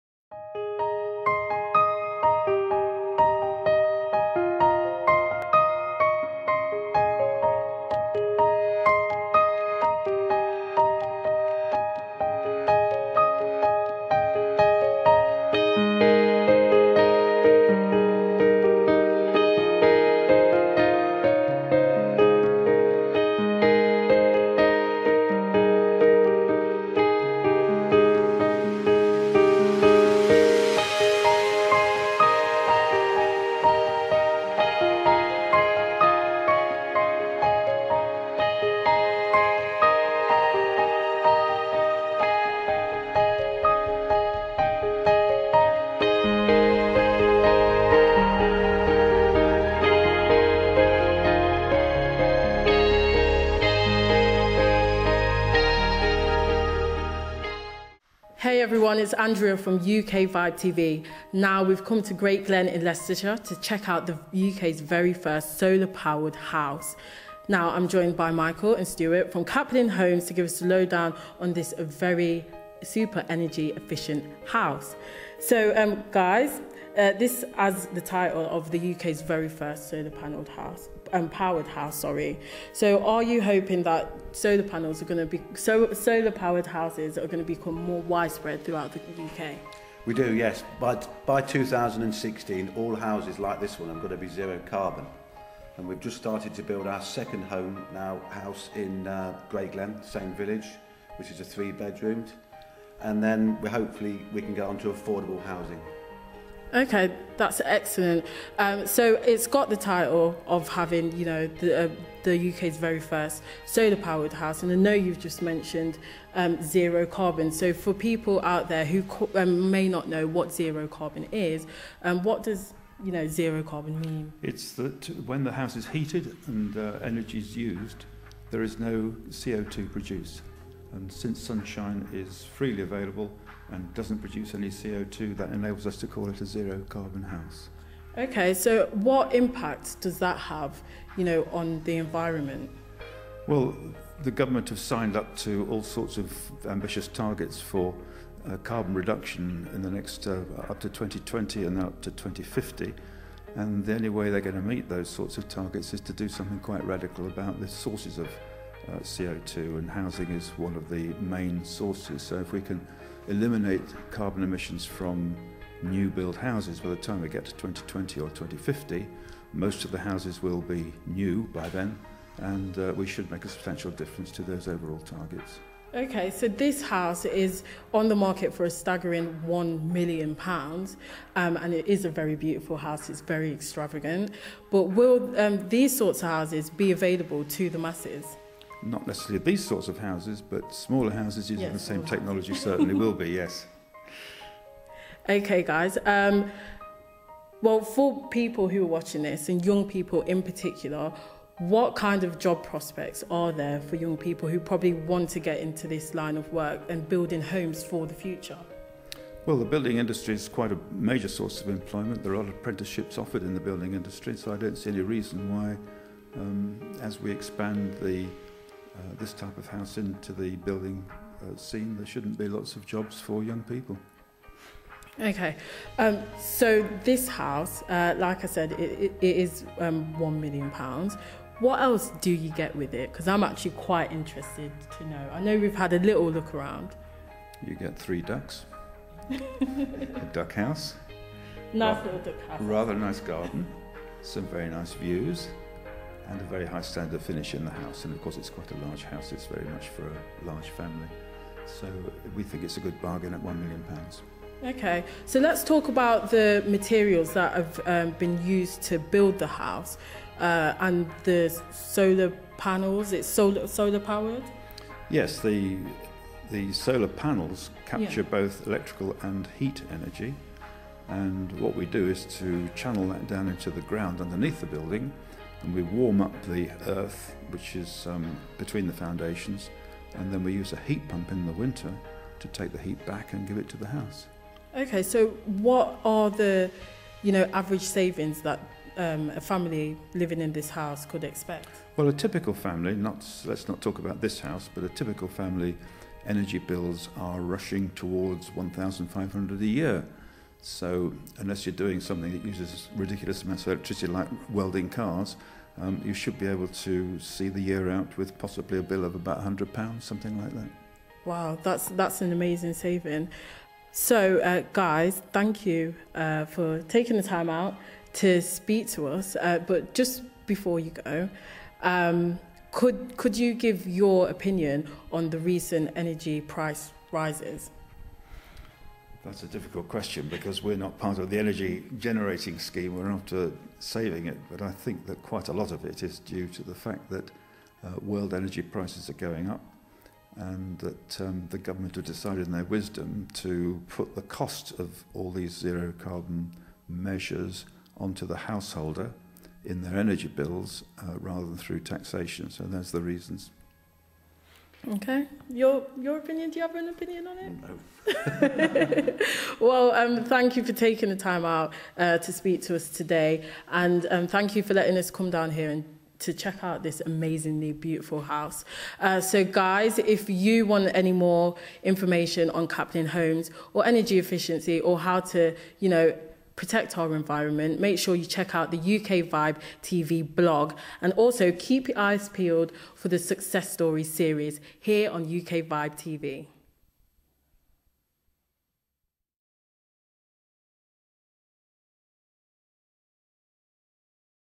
Caplin Homes (Success Stories): UKVibe.TV
The third episode of Success Stories has arrived! We speak to the Directors of Caplin Homes who have built UK's First Solar Powered Home. They chat to us about the Solar House, why electricity bills are rising, what zero carbon is, how much the house costs and much more!

Look out for the next episode of Success Stories!

Want to know more about The Solar House and Caplin Homes?
caplinhomes.co.uk/
solar-house.co.uk/